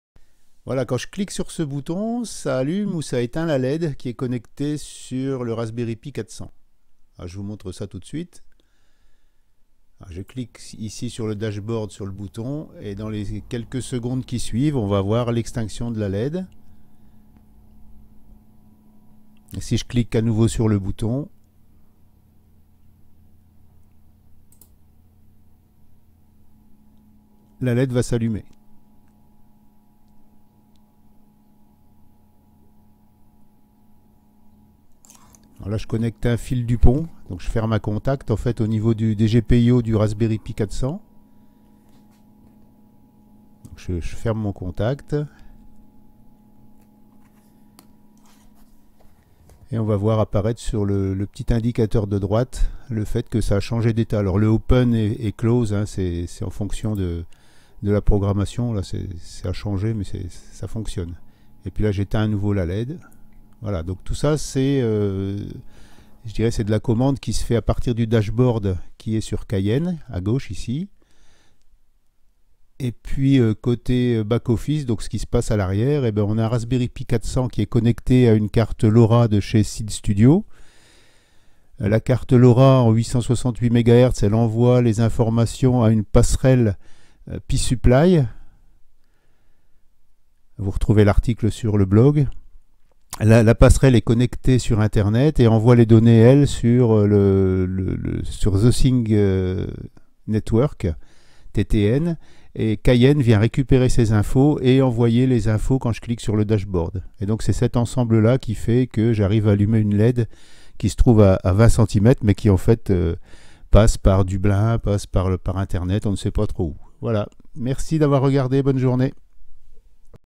Essais de la carte SeeedStudio Grove-LoRa-E5 avec The Things Network et Cayenne
Cette vidéo présente un essai de la carte LoRa E5 de SeeedStudio. La carte envoie les données LoRa vers une passerelle Pi Supply RAK833 qui les transmets à The Things Network (TTN). Une connexion Cayenne permet de visualiser l'état des capteurs et d'actionner une LED à Distance.
Cette vidéo fait partie d'un article à paraître bientôt sur le blog framboise314.fr :